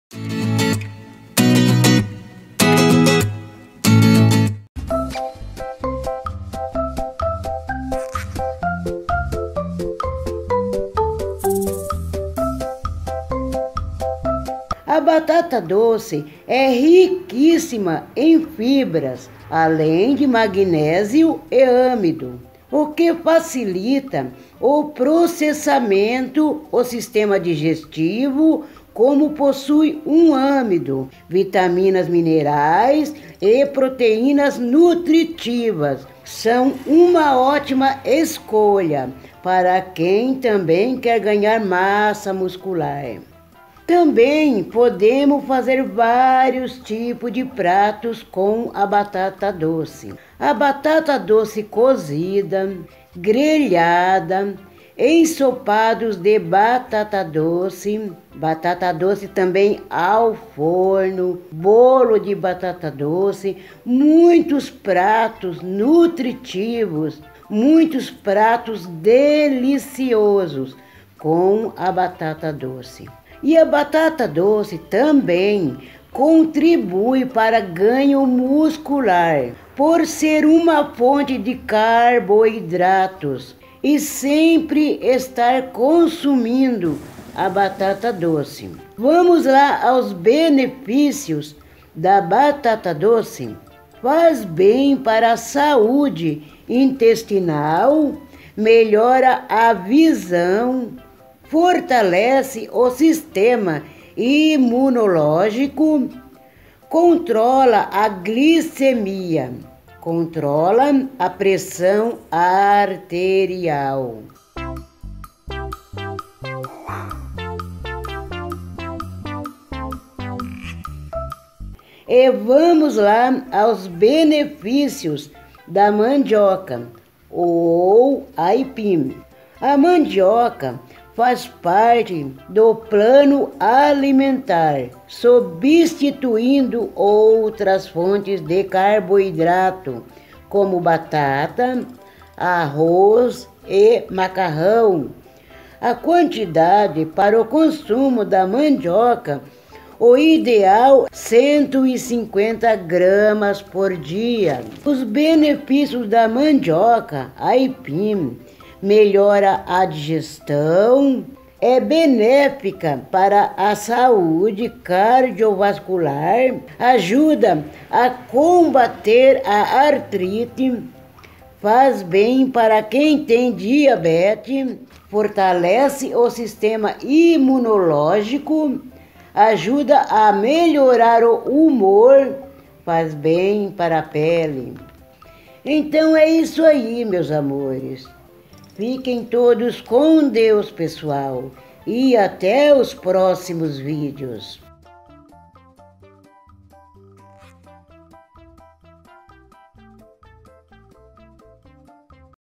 conheçam alimentos que cuidam do corpo e curam doenças e quais são eles?
OIE  meus amores  bom dia ,boa tarde, boa noite, tudo bem com vocês?
hoje vou estar trazendo os benefícios da batata doce e da mandioca aipim,
vamos consumir mais vezes esses tipos de alimentos?